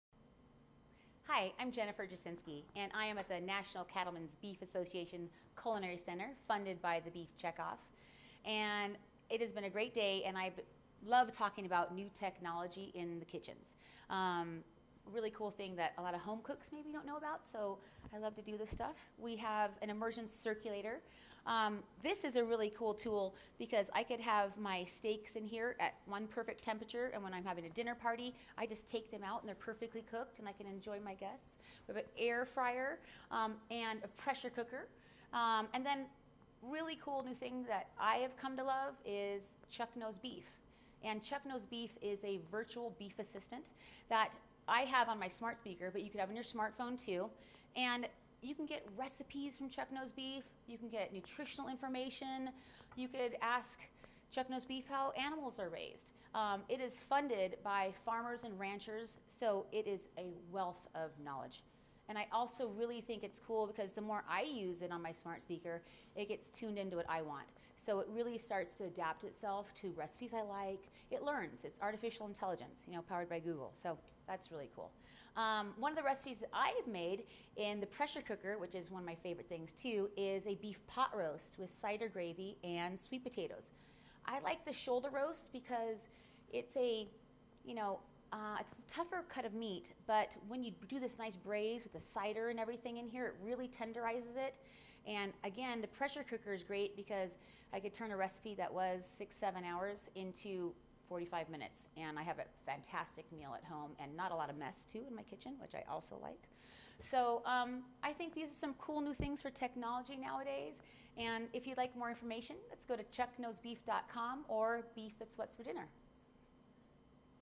Meet Chuck Knows Beef
Beef goes virtual as the iconic Beef. It’s What’s For Dinner. brand brings the knowledge of a rancher, the expertise of a chef, and the humor of a dad into the kitchen with the official launch of Chuck Knows Beef, a new beef virtual assistant. Chuck can be easily accessed at ChuckKnowsBeef.com or by simply enabling Chuck Knows Beef with Amazon Alexa or Google Home Assistant devices.